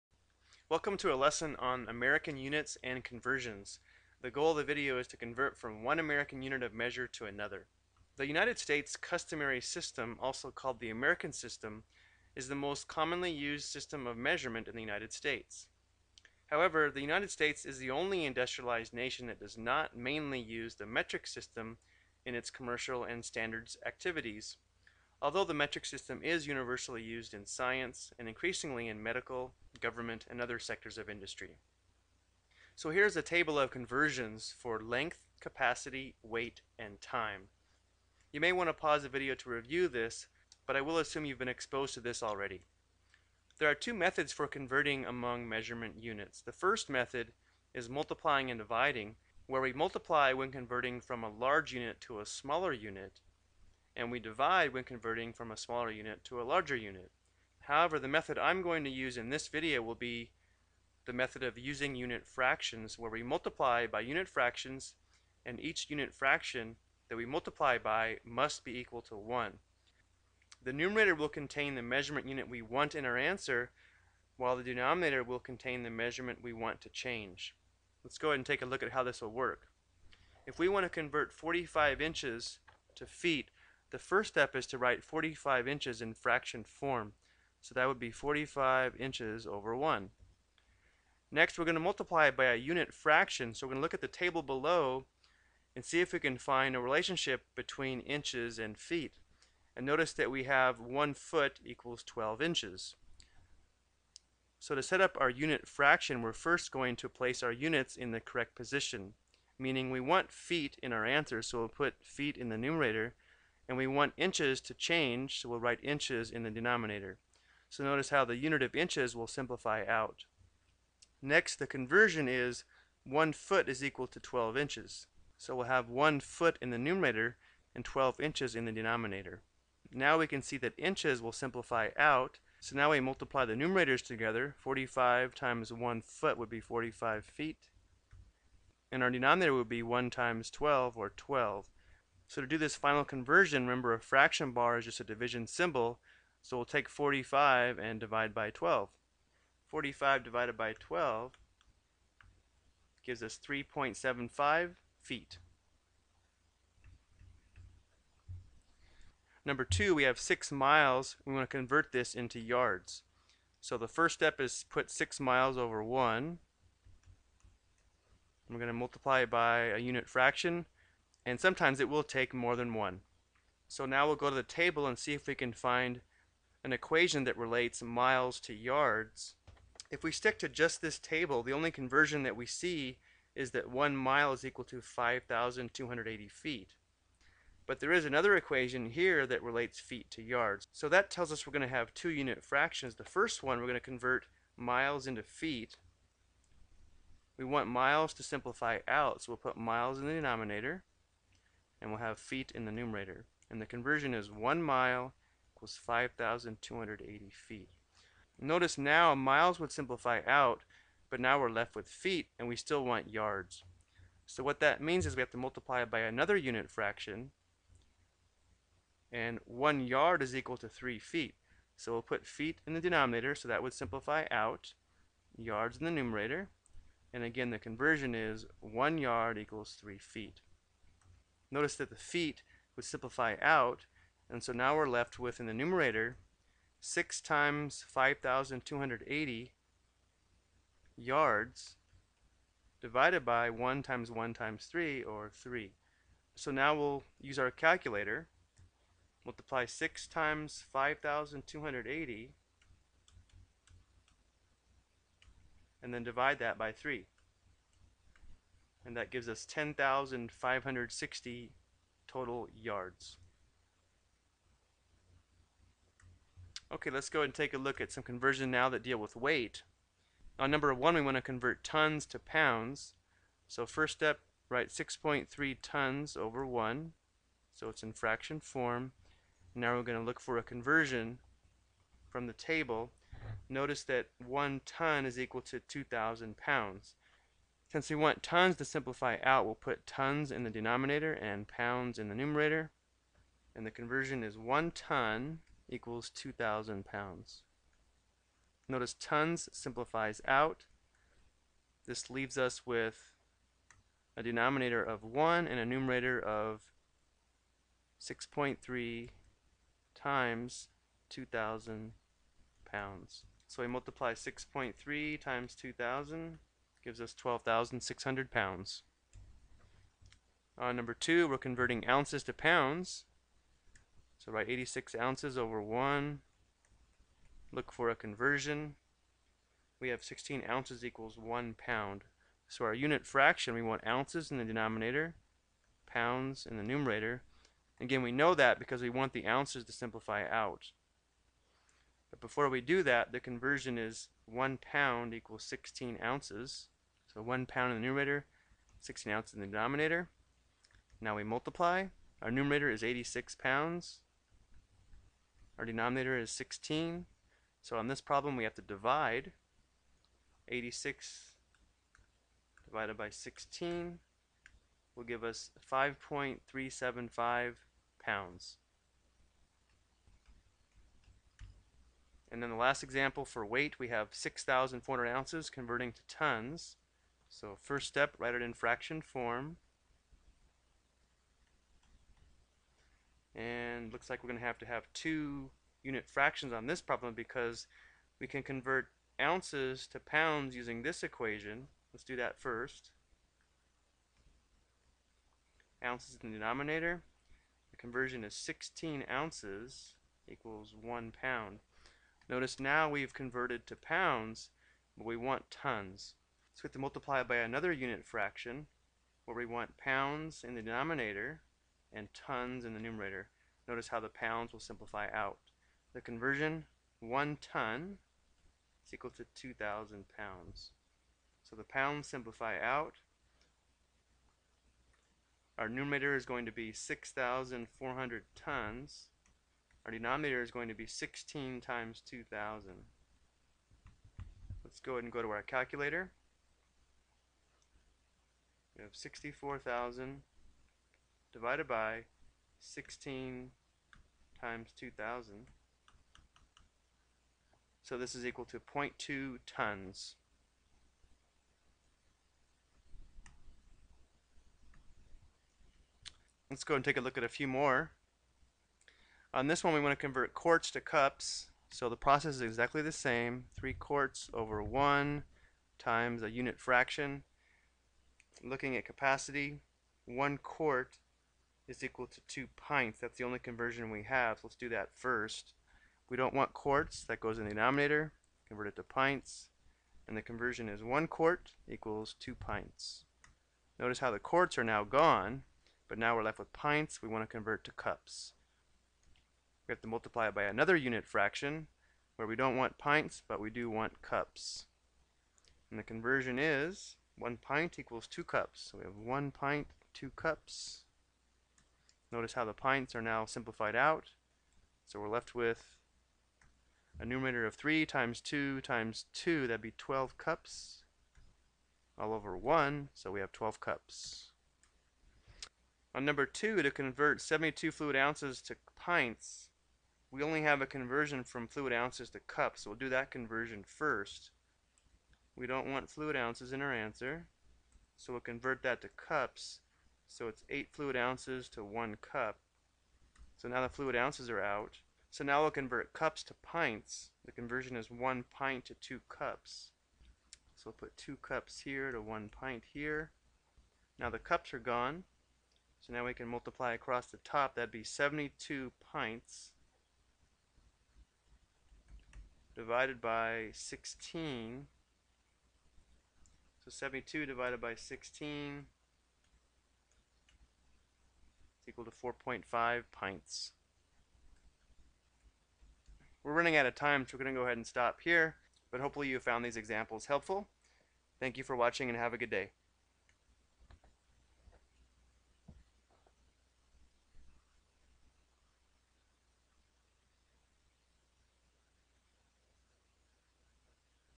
American Unit Conversion
TeacherTube User: Bullcleo1

This video explains how to convert between different American or Standard unit of measure for length capacity and weight.